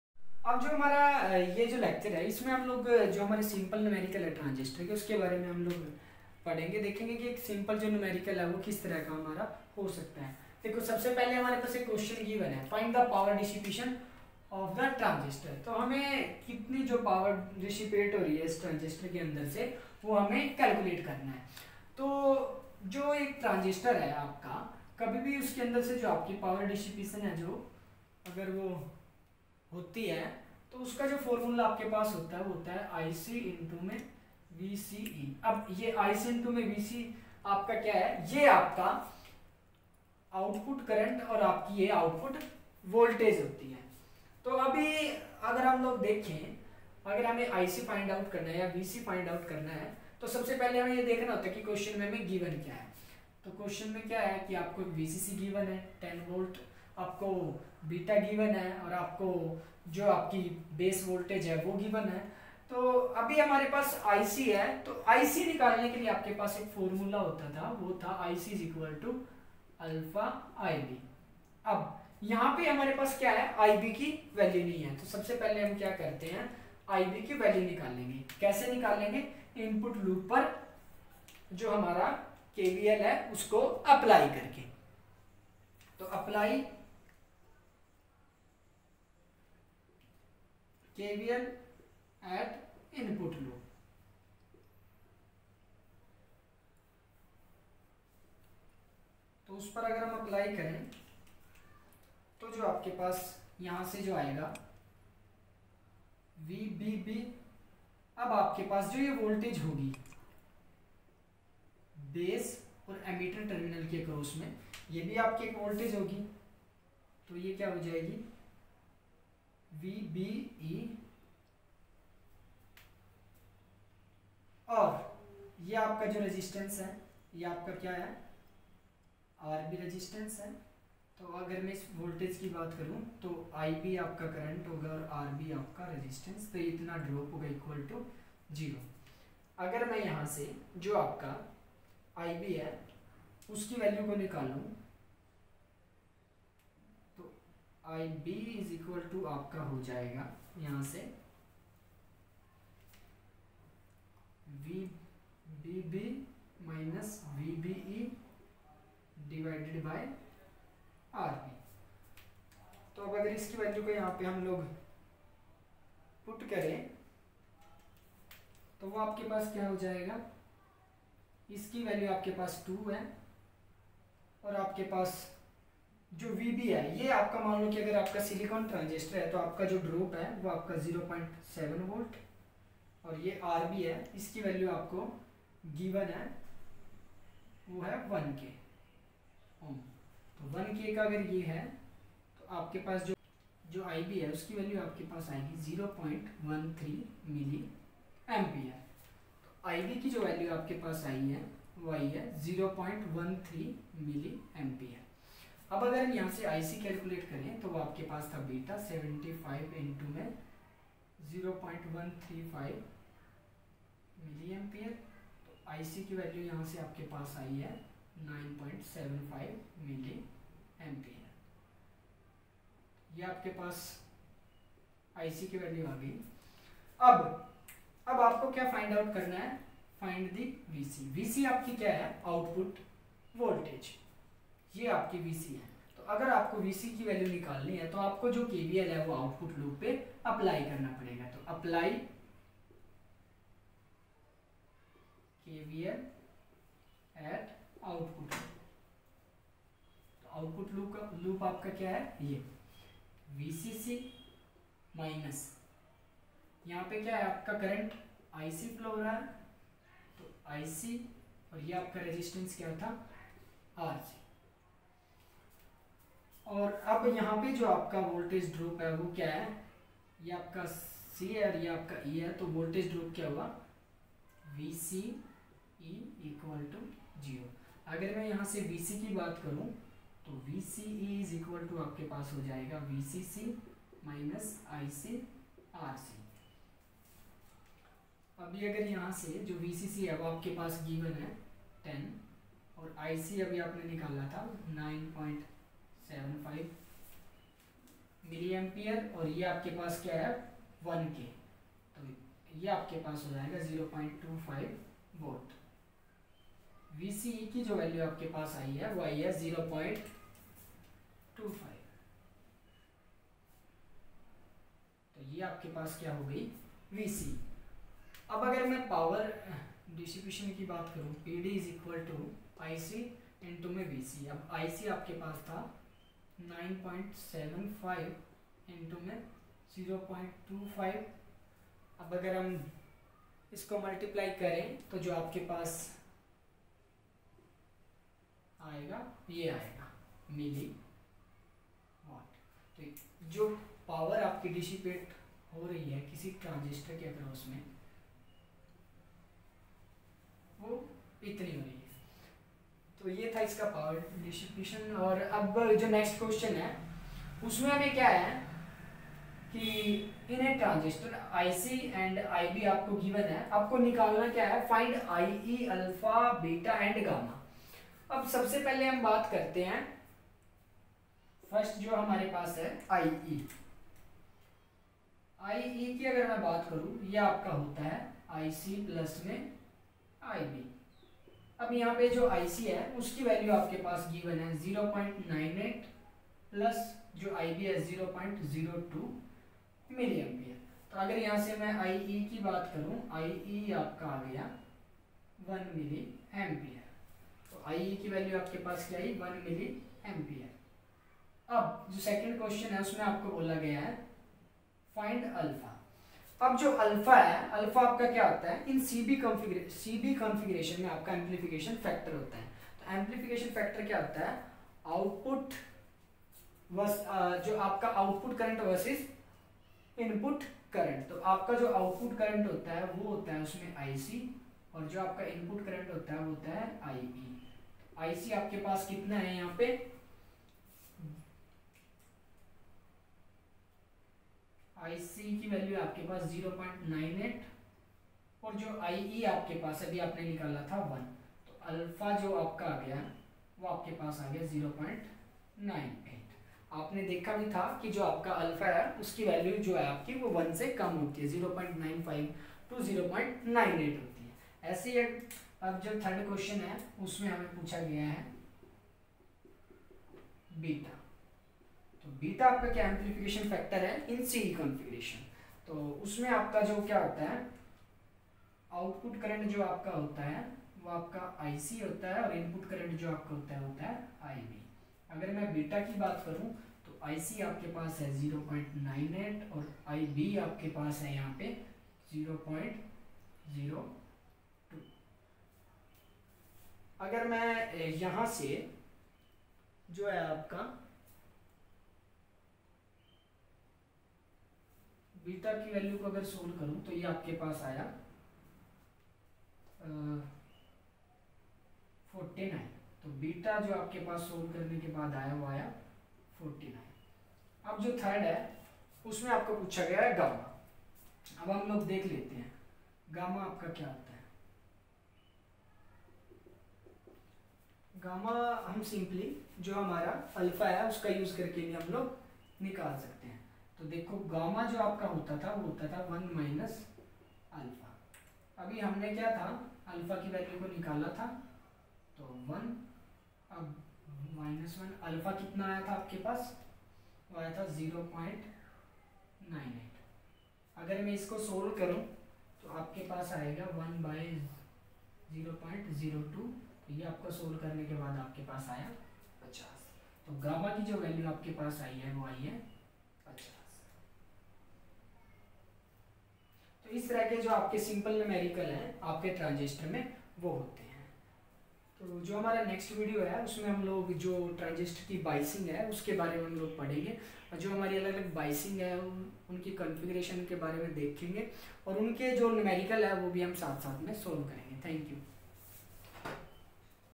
Numerical solution of transistor in Hindi || analog Electronics || by ajit singh
transistor work as a switch
transistor work as a amplifier
conduct by-Ajit singh